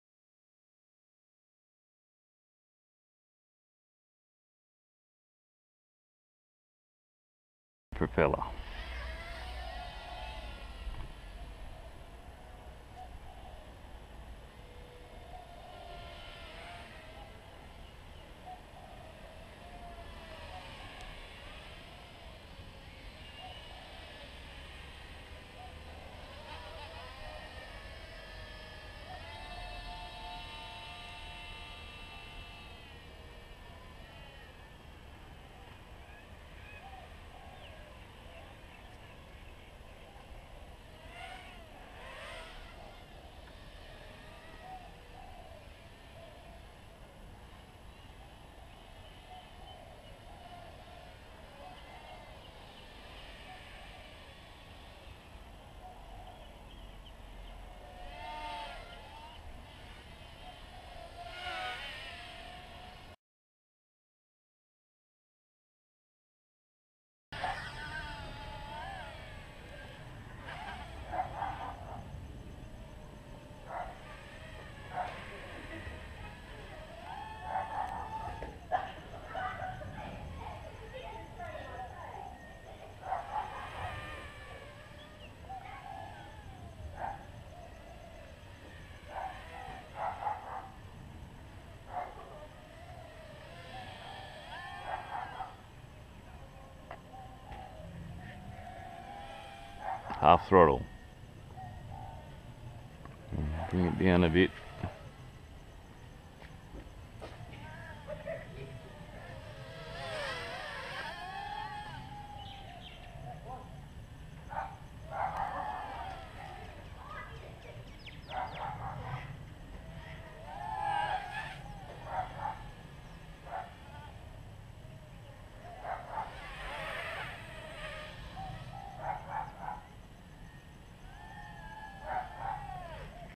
Nano QX Motor Upgrade
Upgraded motors on my Blade Nano QX. Got the motors from Micromotor Warehouse